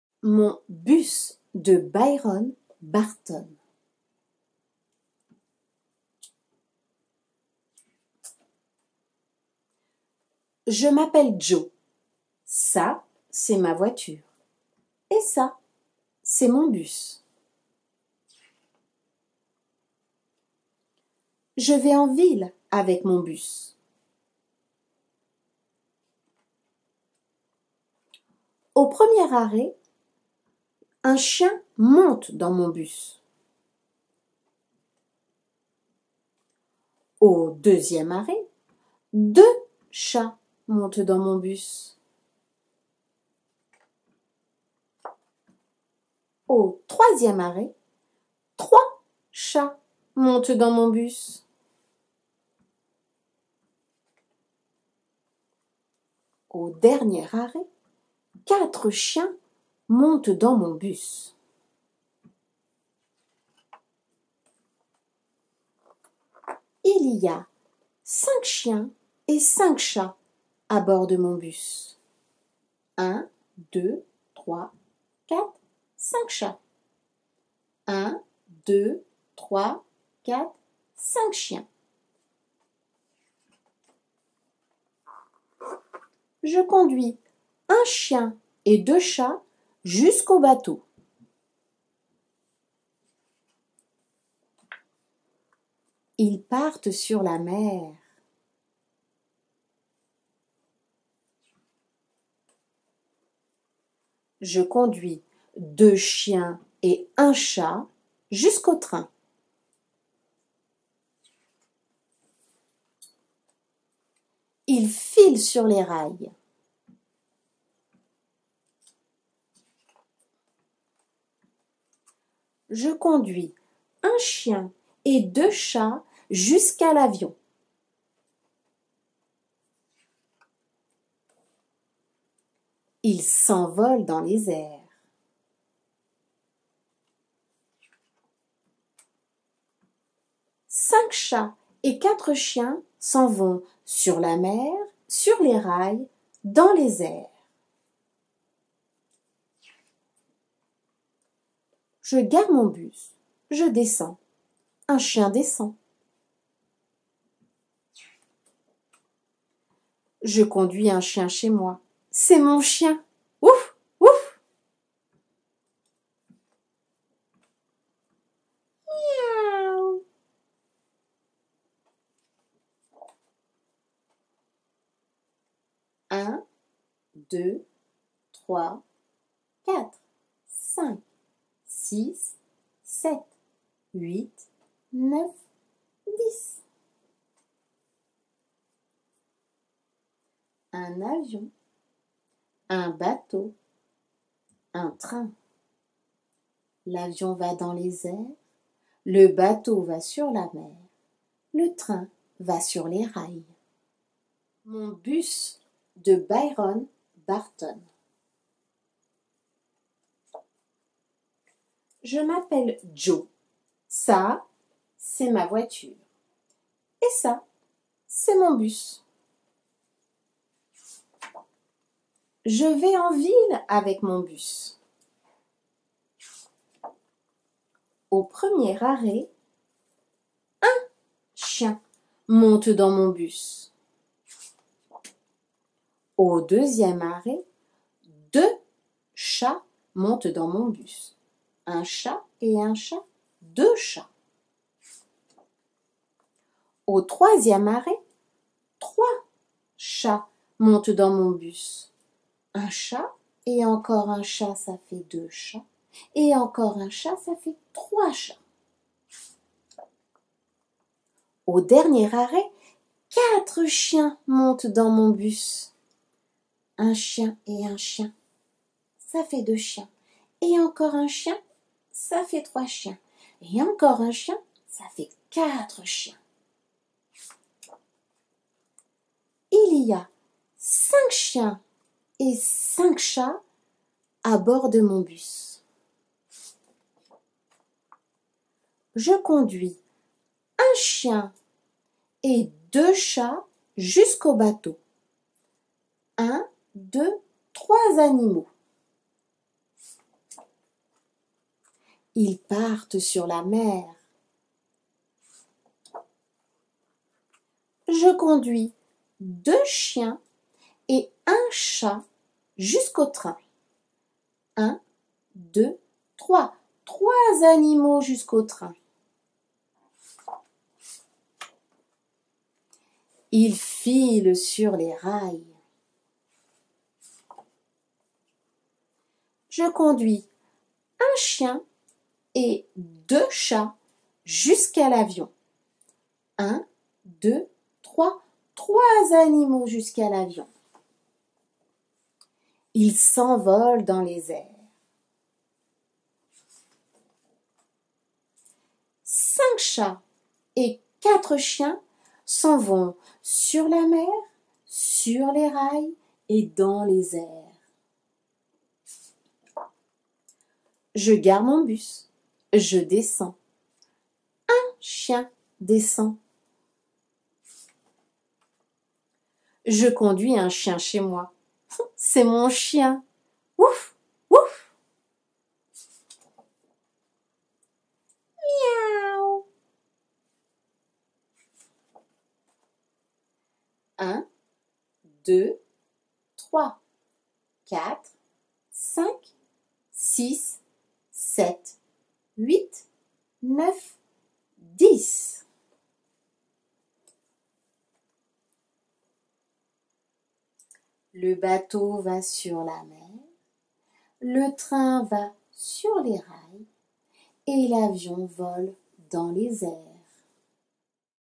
Lecture offerte - Mon bus de Byron Barton
Deux lectures de cet album de Byron Barton s'enchaînent. La première, classique, est une lecture globalement simple, qui permet de comprendre l'histoire, de se concentrer sur le vocabulaire. Lors de la deuxième lecture, je m'attarde plus sur le côté mathématiques de ce livre... en ajouter les quantités sur les doigts et en dénombrant le nombre d'animaux qui montent dans chacun des moyens de transport : ainsi 1 chien et 2 chats, ce sont 3 animaux...
Bonne écoute les enfants !